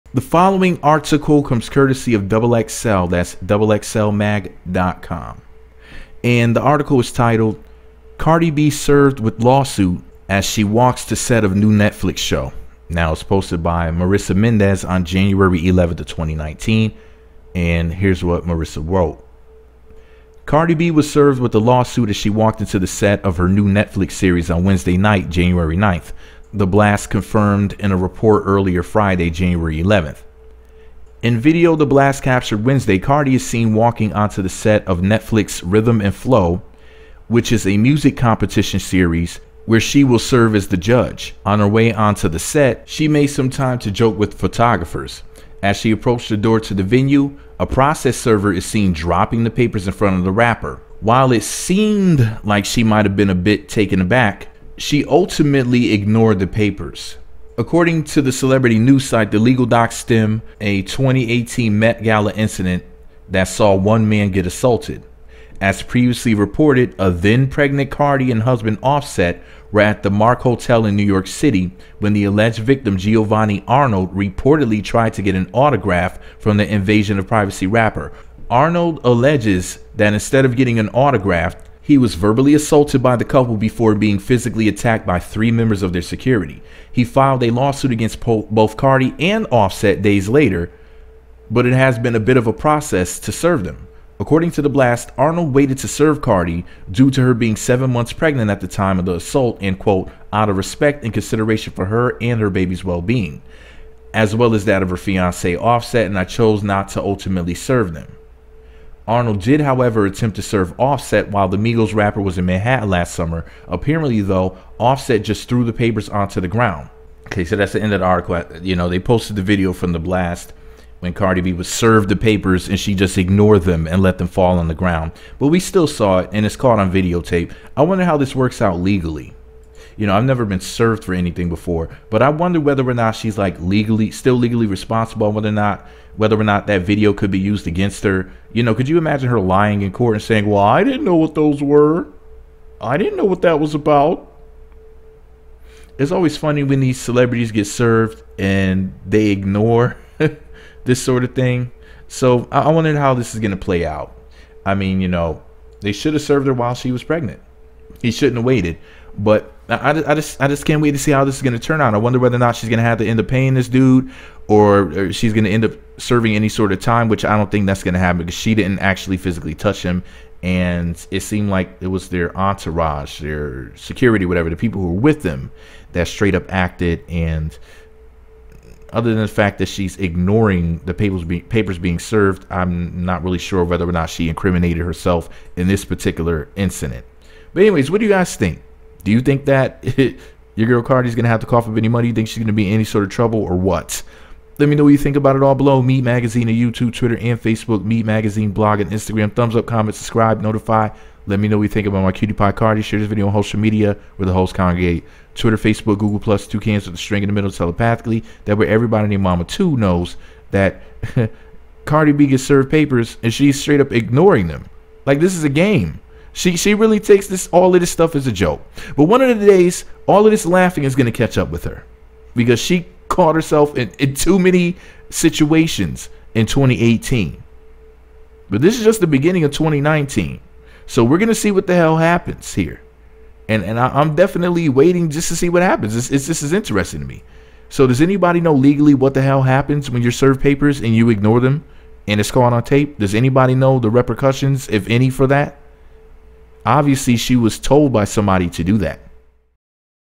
Cardi B served papers over Met Gala fight! Cardi ignored servers papers at RhythmandFlow
Cardi B served papers over Met Gala fight! Cardi ignored servers papers at Netflix show Rhythm and Flow! Love and Hip Hop New York Season 7 cast member Bardi wont accept the lawsuit documents! She ignored them, she clearly saw the server. I wonder how this is all going to end!